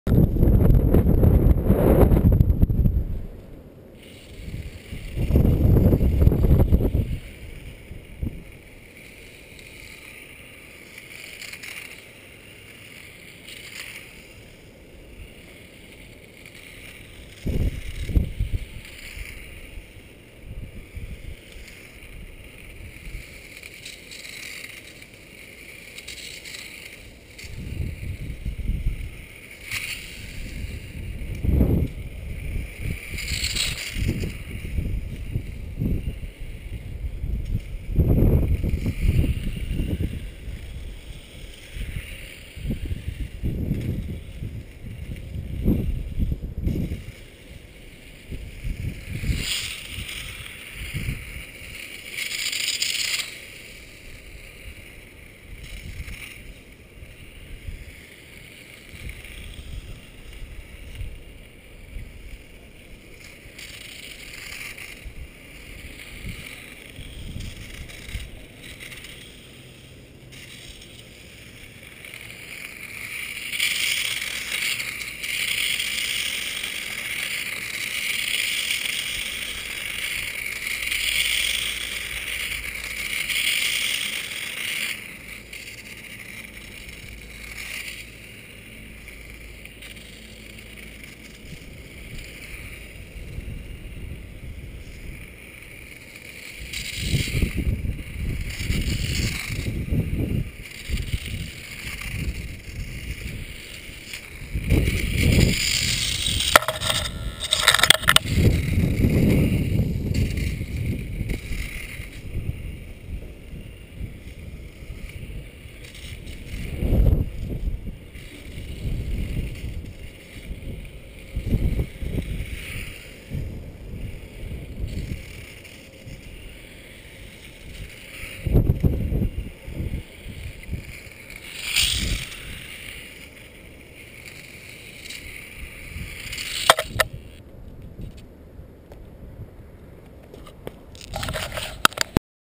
Silverlit Ducati RC
fun little RC bike that knee drags